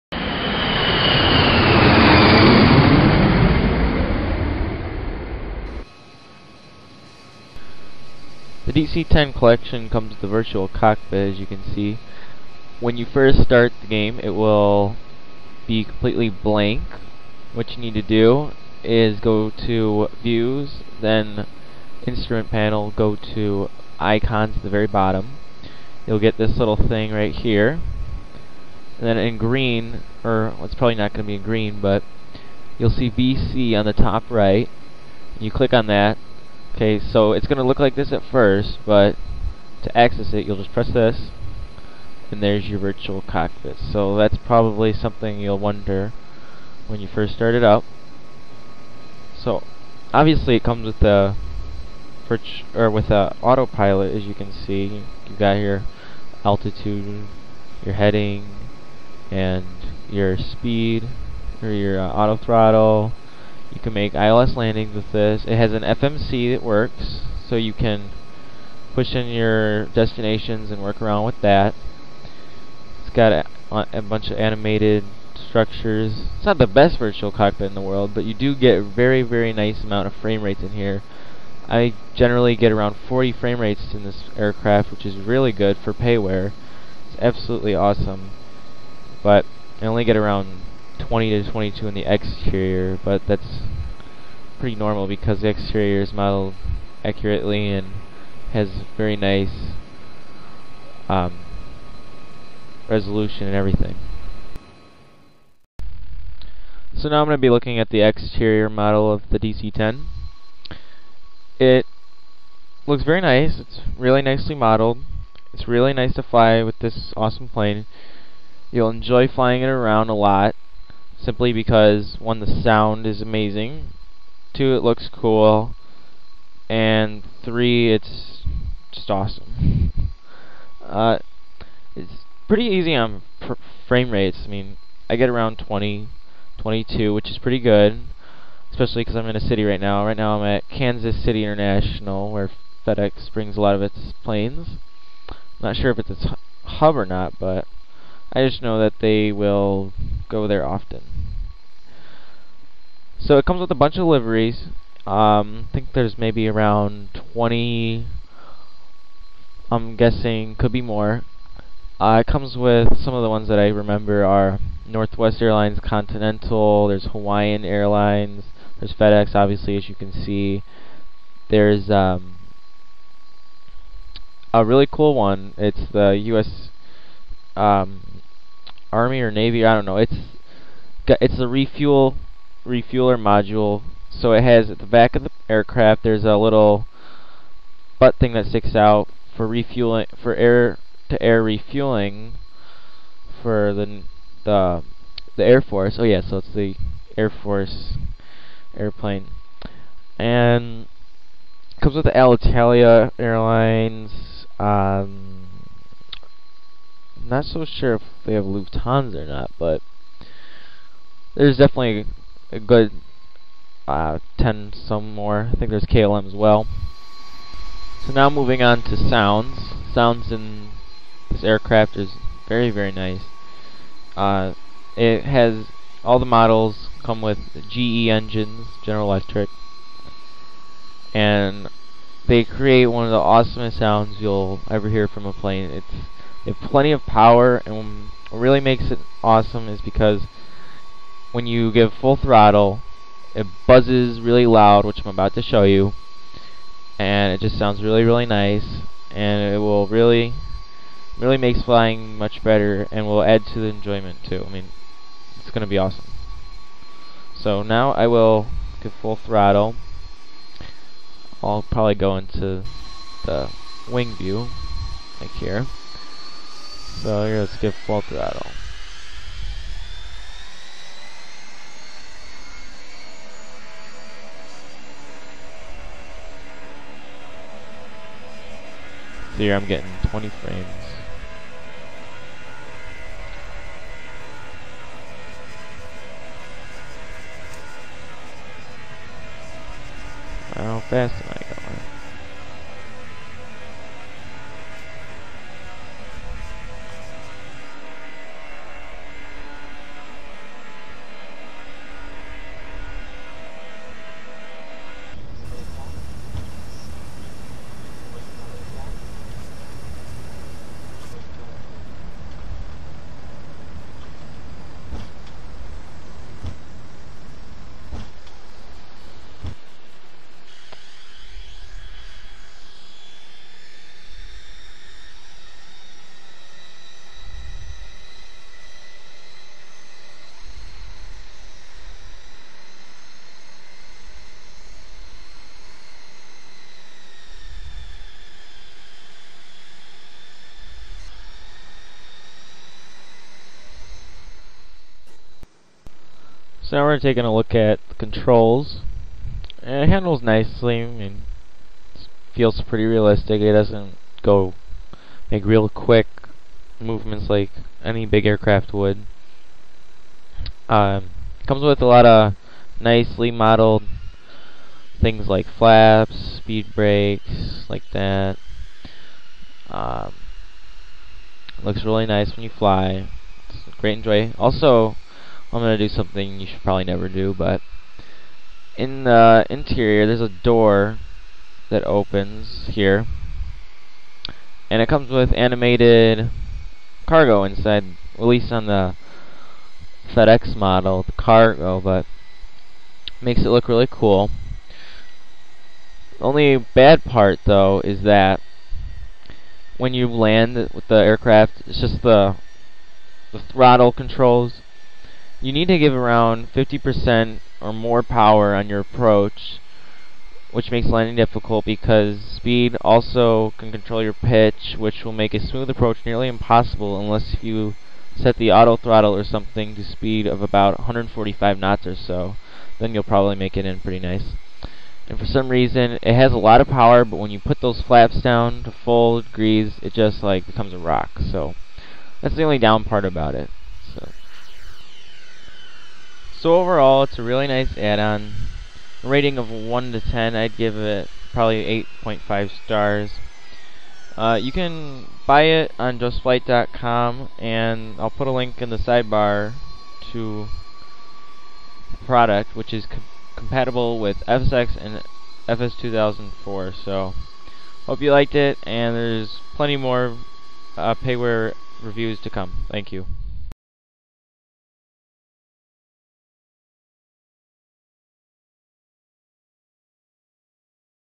Just Flights/CLS DC-10 Collection review
This is a review of a great aircraft called the DC-10. The aircraft has been put out of service, but still flies today mostly as a cargo transporter.  I recommend you buy this plane if your looking for a nice high quality payware add-on thats not to hard on your computer.
The product is available here and is not going to empty your pocket either